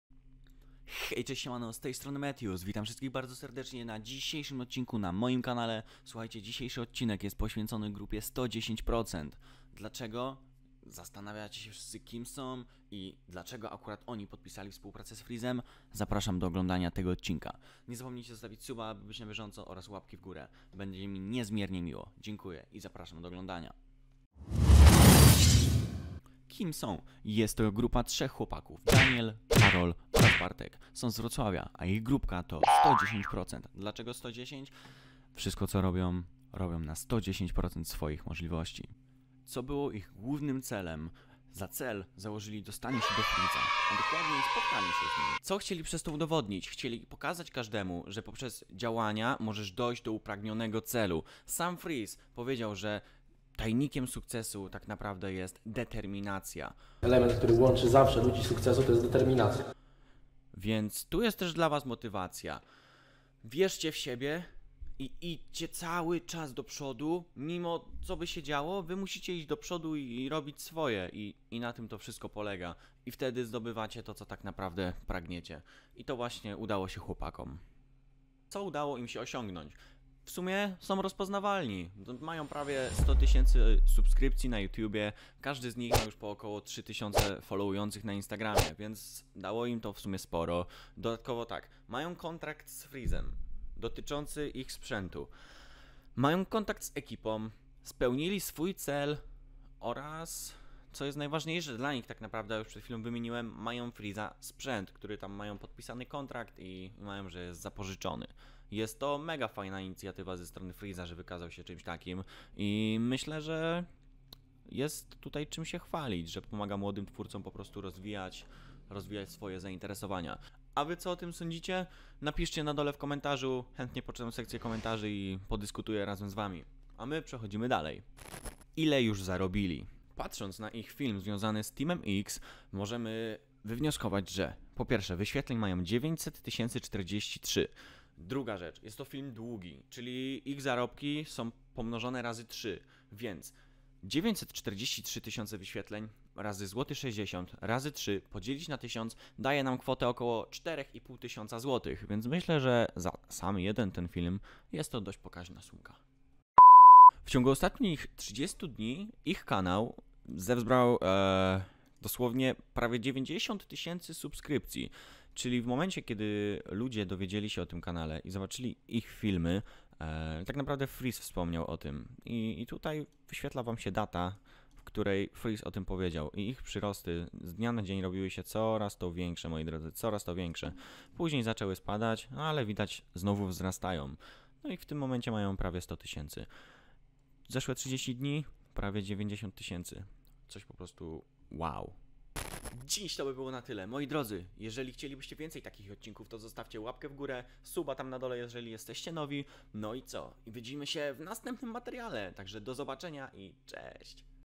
FRIZ i 110% O CO CHODZI | MeTiusOfficial
Siemano, film wyjsniający oo FRIZIE i 110%
ZAPRASZAM DO OGLĄDANIA

                                                              (MeTiusOfficial)
🎬 Streamuję: Open Broadcaster Software
🎥 Nagrywam twarz:  Vlogi (Samsung S8+) / Stream (Logitech c920)